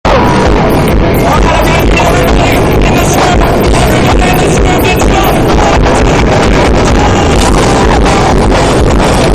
SK is crazy!!!
This is a short video of what it's like to be 1st row at a Stellar Kart concert. Enjoy!! =]